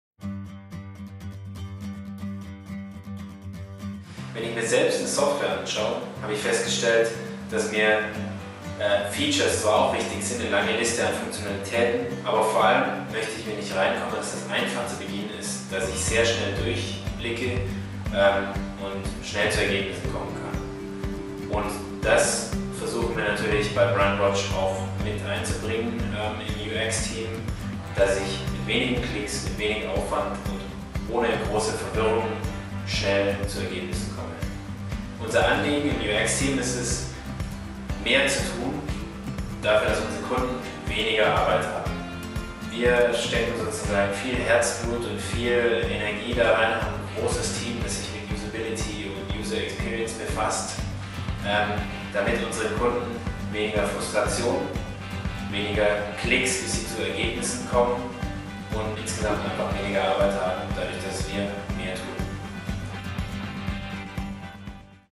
Mehr Freude an der Nutzerführung, weniger Verwirrung
Julius Dietz, Geschäftsführer von Brandwatch in Deutschland, im Gespräch über das User Interface des Social Media Monitoring Tools.